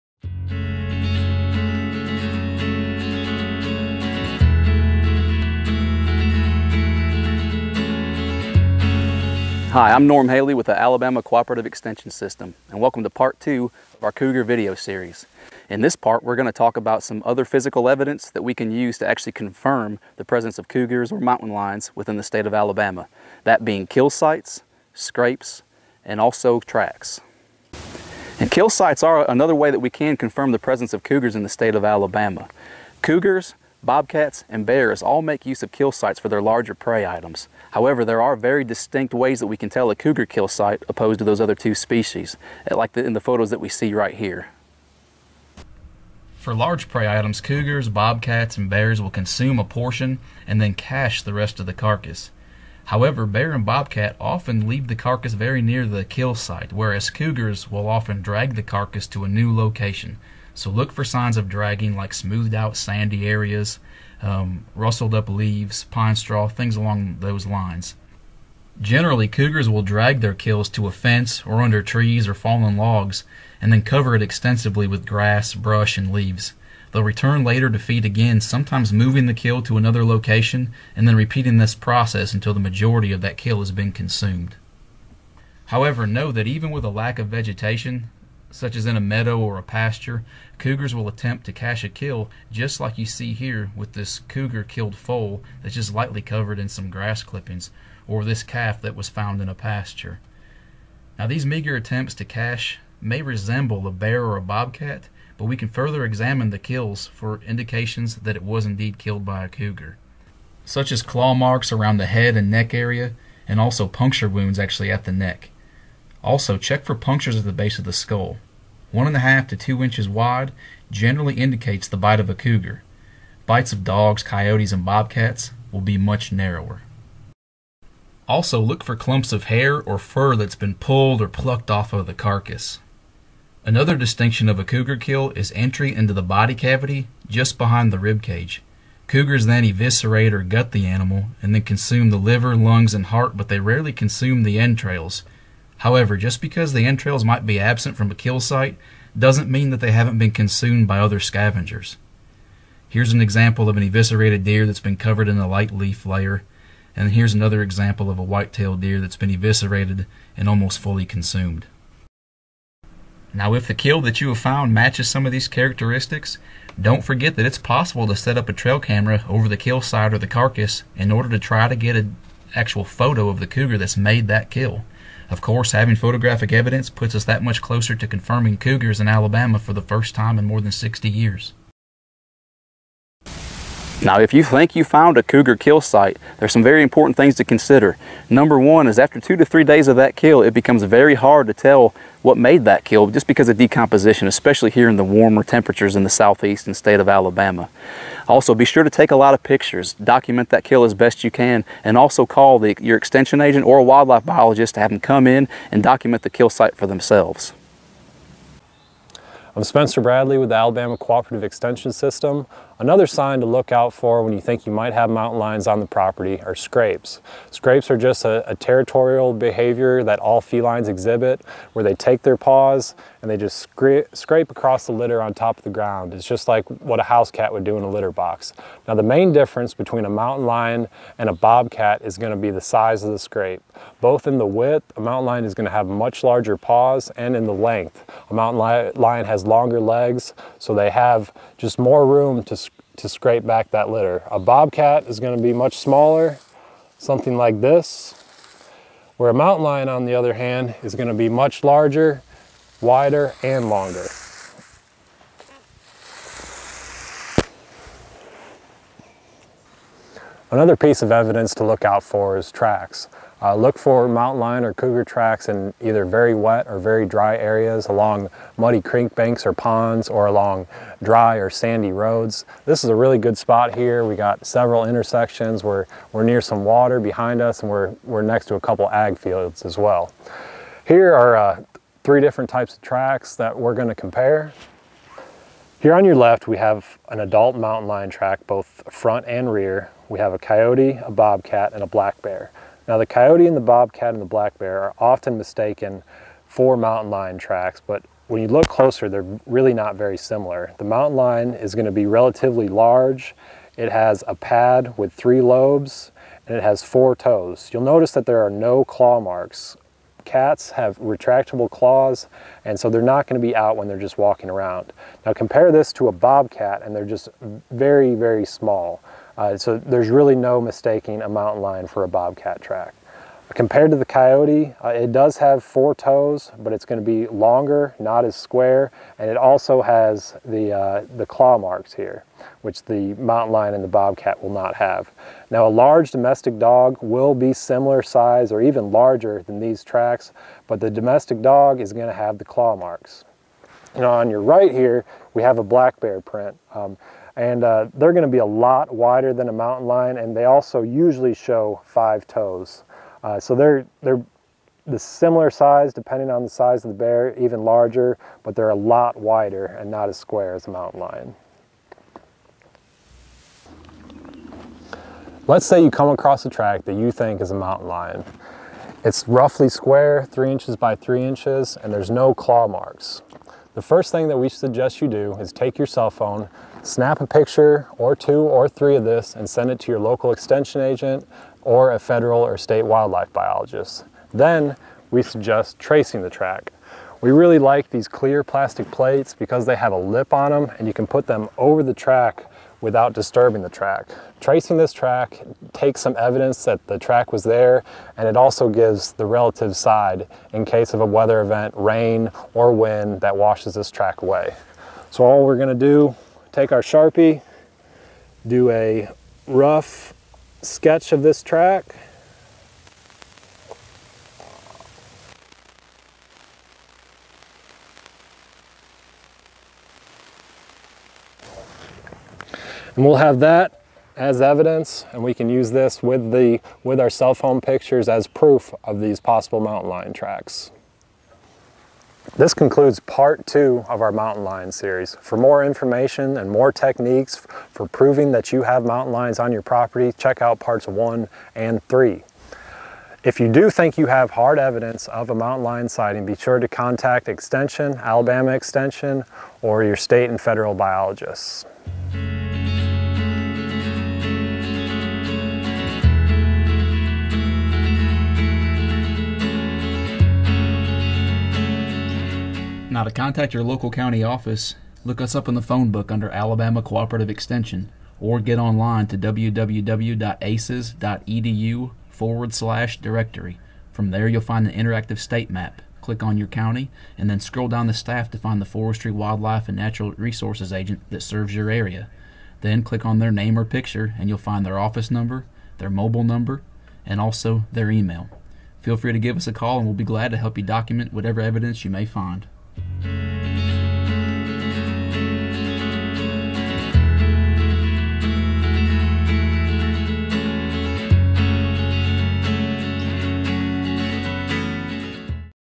Part 2: Kill Sites, Scrapes, & Tracks
In part-2 of this 3-part series, extension agents from the Alabama Cooperative Extension System guide you through ways to identify the sign left by cougars and how to distinguish that sign from other species.  With a little training and awareness, the kill sites, scrapes, and tracks left by cougars can easily be determined.  If you feel you have hard evidence of a cougar in the state of Alabama, please contact your local county extension office by visiting our directory